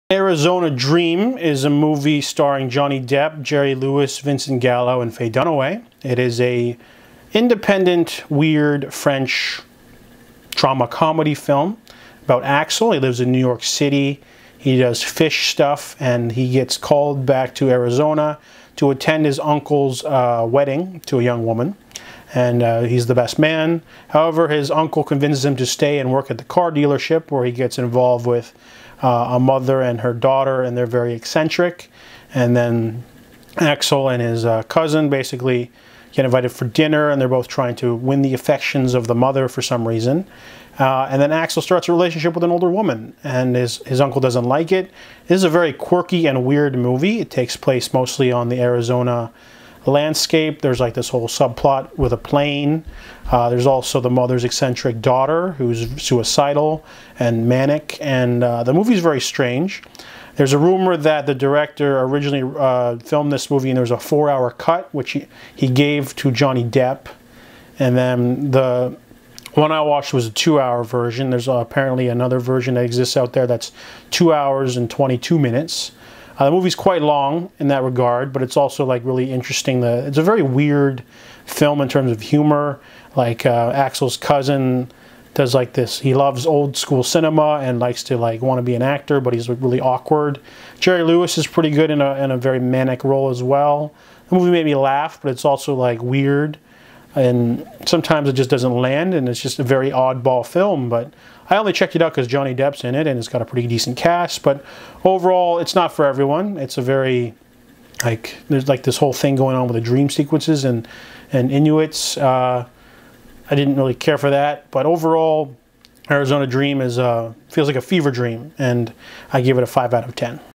MovieFiendz Review: Arizona Dream (1993)
A young adult goes to Arizona to attend his uncle's wedding, but gets roped into staying and falls in love with an older woman.